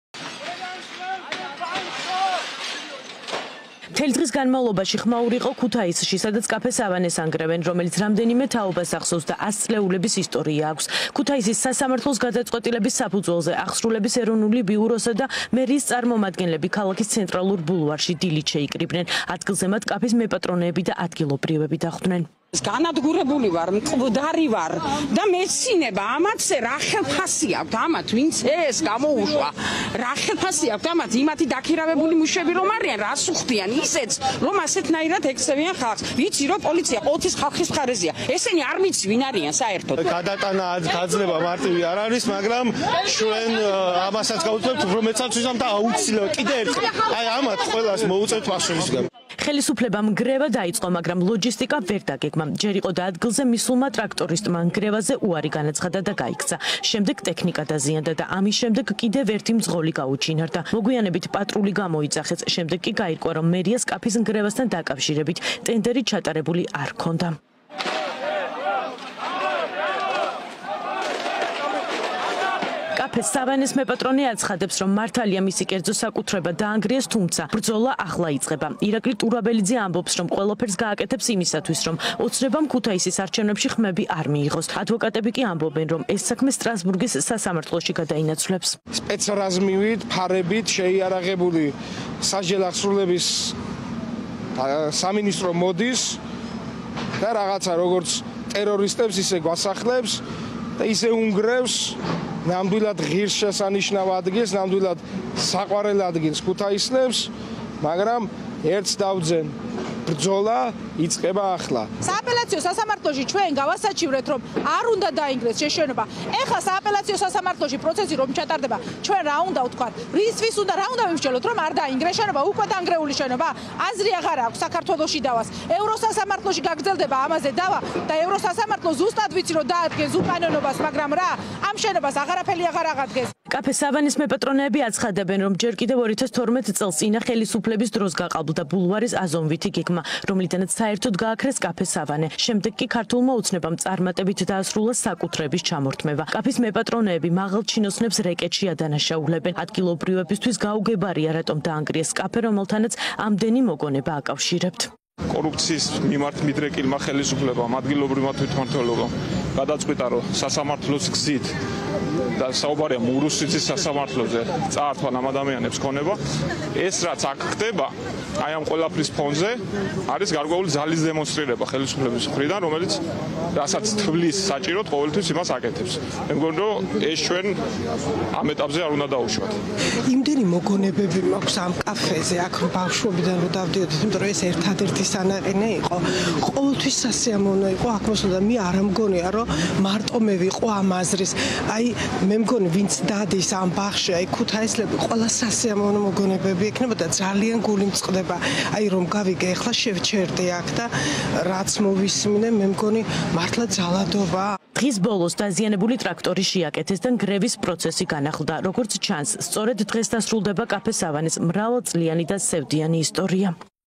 ქუთაისში კაფე „სავანე“ დაანგრიეს
▪️ ქუთაისში, ისტორიული კაფე „სავანე“ დაანგრიეს — მესაკუთრეების თქმით, დემონტაჟი გაყალბებული დოკუმენტაციის საფუძველზე მოხდა